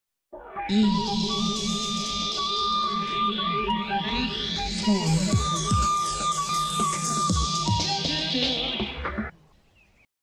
Badgers and Chickens and Pigeons, Oh my!
The first version of my finalised ident. A later version may appear with sound effects and refined animation, but I ran out of time because I was pissing about with the details so this'll do. It's not very good but I'd barely used After Effects at this point so whatever.